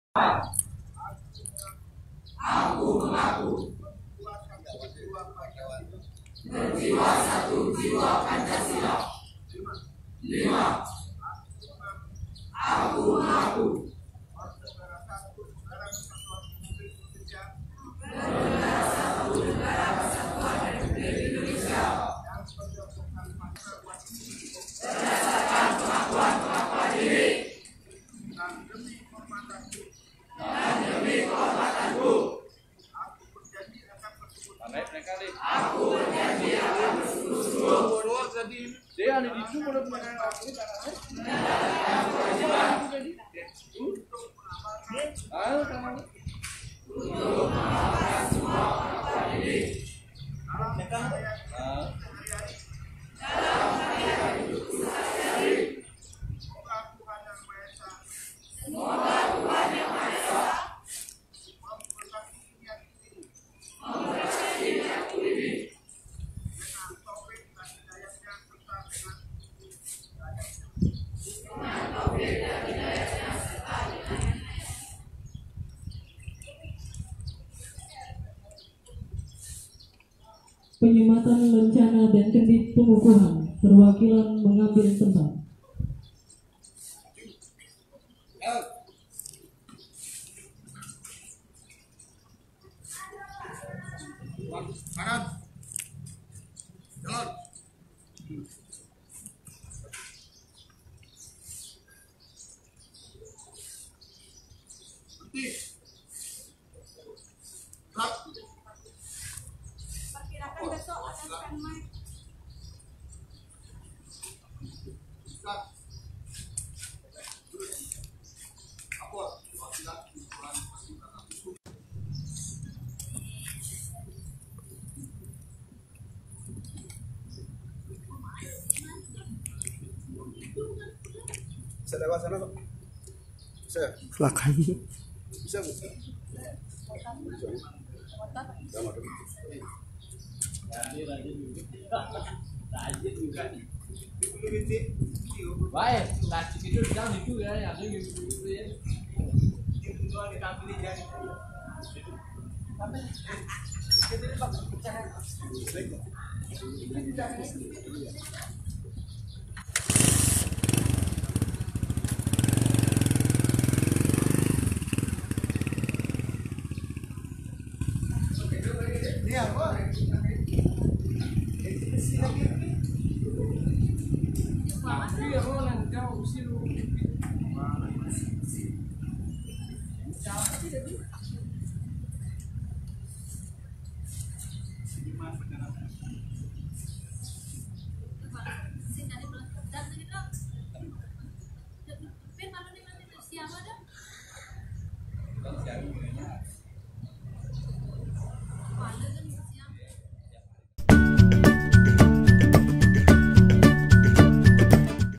Gladi Bersih Paskibraka Persiapan HUT ke-73 Republik Indonesia
Gladi upacara bendera hari peringatan Kemerdekaan oleh Paskibraka, di halaman Kantor Bupati Klungkung, Selasa (13/8/2019).